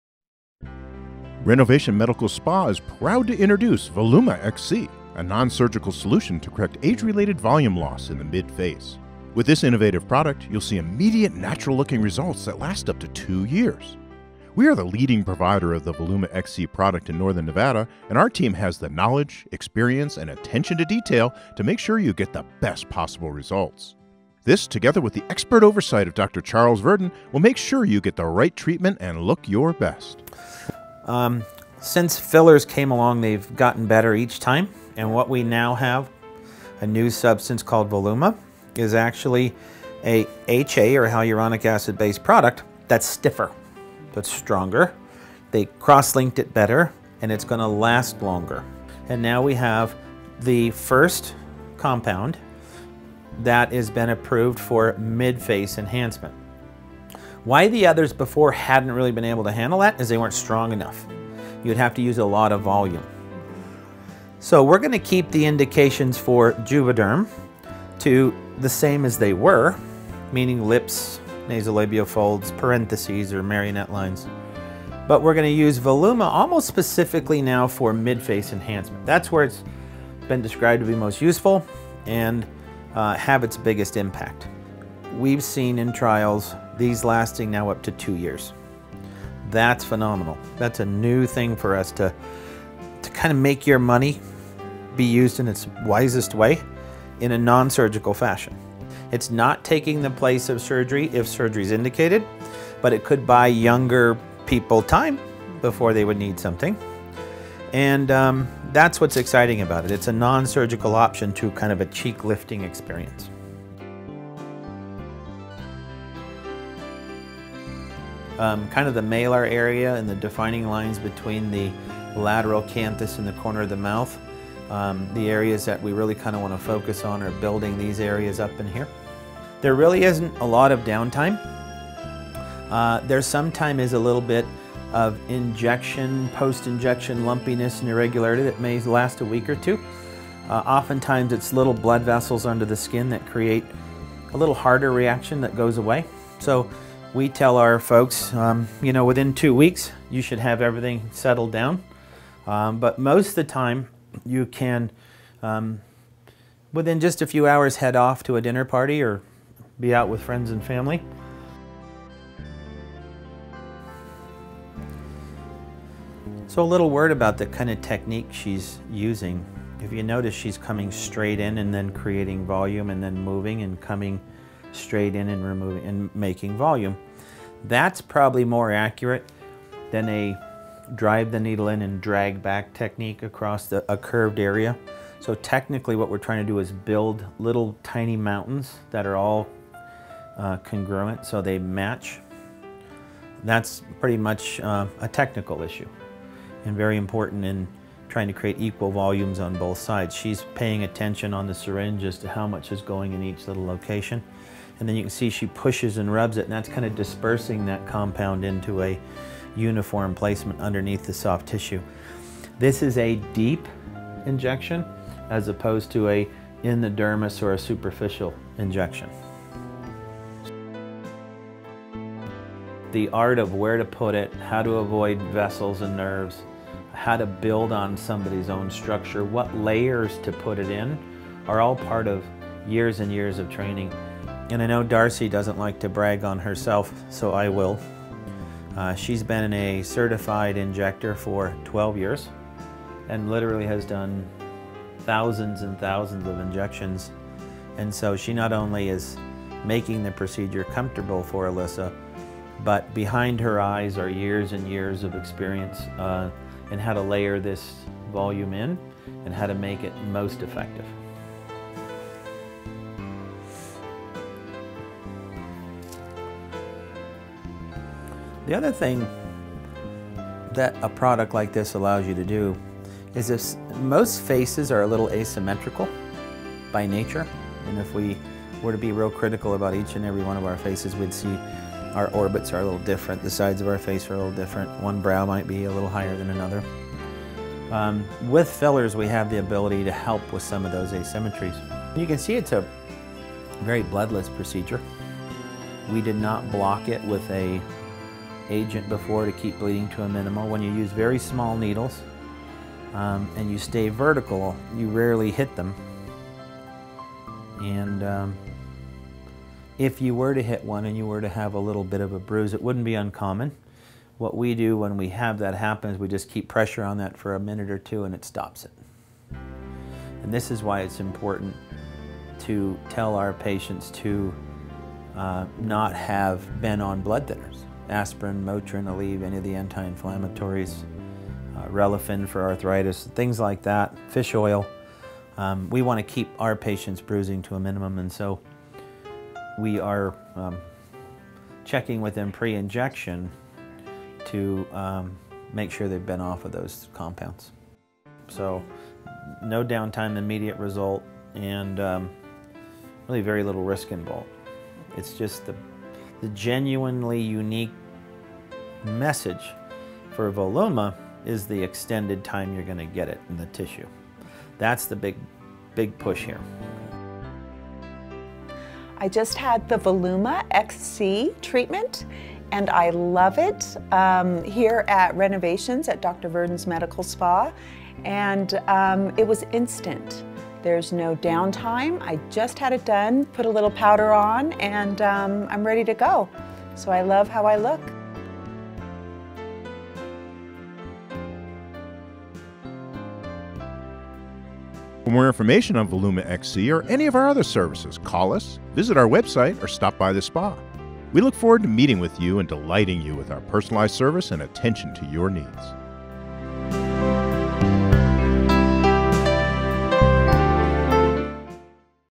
Introducing JUVÉDERM VOLUMA™ XC in Nevada at Renovation Medical Spa
Dr. Virden, a board-certified plastic surgeon in Nevada uses JUVÉDERM VOLUMA XC in 3 cheek areas, and the effects of a treatment can last up to 2 years.

In this video, Dr. Virden introduces JUVÉDERM VOLUMA XC to a patient at Renovation Medical Spa in Reno, NV, and explains the differences between this hyaluronic acid gel and other dermal fillers, and the benefits of VOLUMA XC in the 3 cheek areas of patients who have lost check volume due to aging.